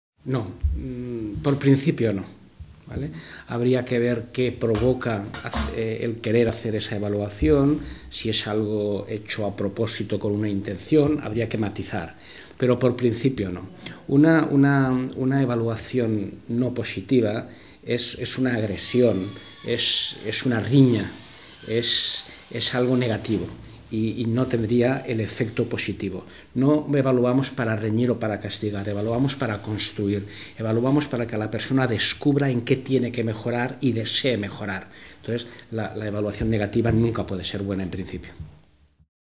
E01 Una evaluación que no es positiva (constructiva) ¿es buena?
Una evaluación que no es positiva (constructiva) ¿es buena?

responde las preguntas de los participantes a las Jornadas Pymes 2010 en Buenos Aires, donde presento la conferencia Aumento de la Productividad

InfoEmpresario.com  Claves  y respuestas para Directivos de Pymes